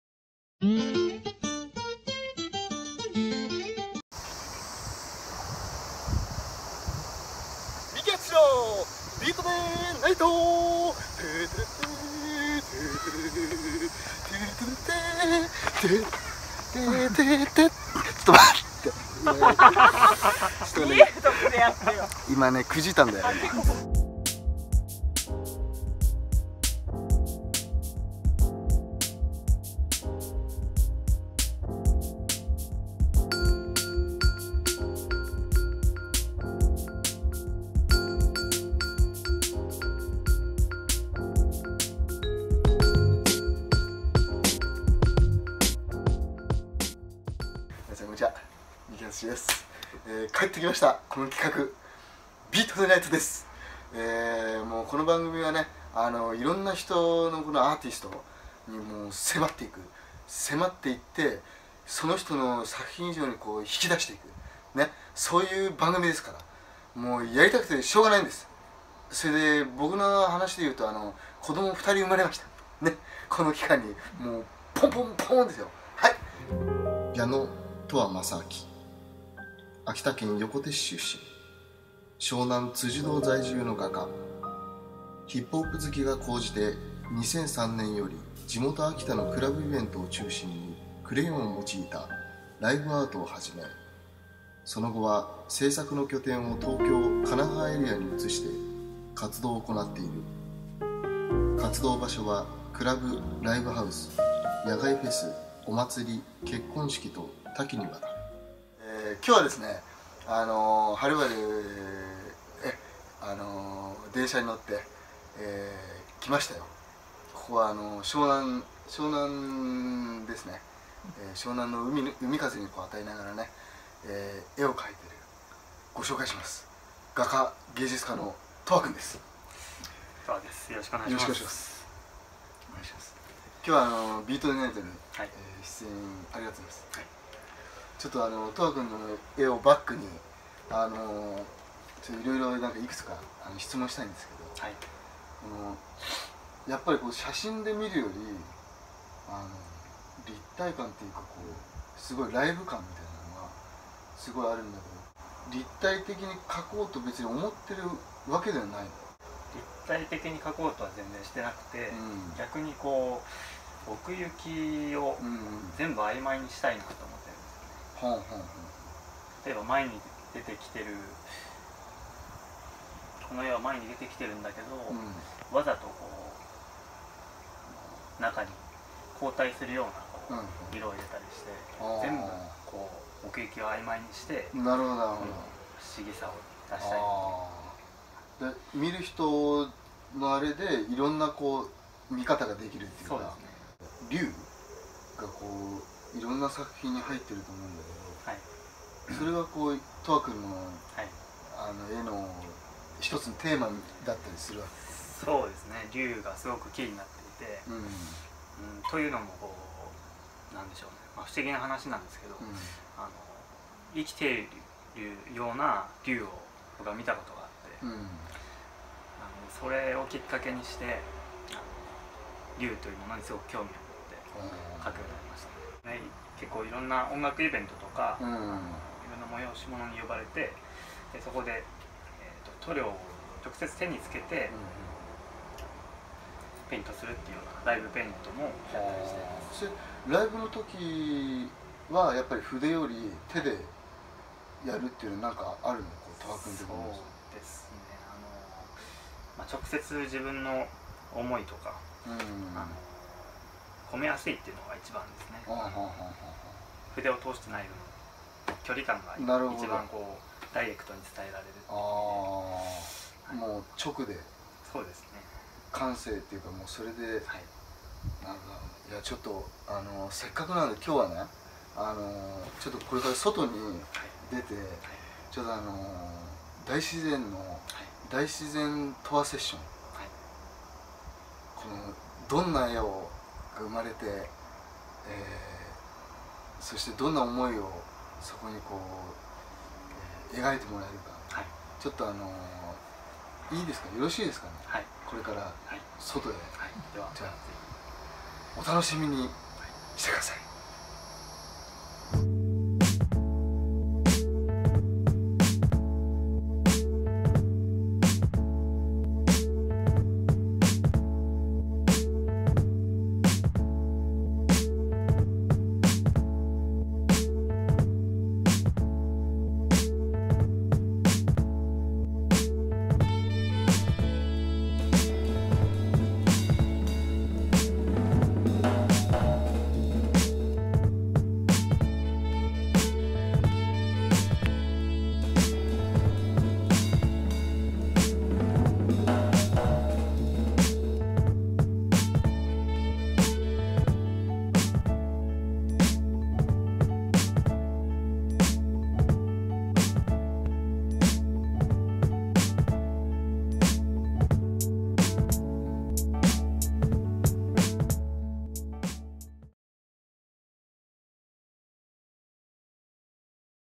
『みきあつしの Beat De Night』#9 TOWA
ドラマーみきあつしがお送りするYouTube番組！様々なアーティストをゲストにお迎えする。9のゲストは画家の矢野''TOWA''正晃さんを迎え放送。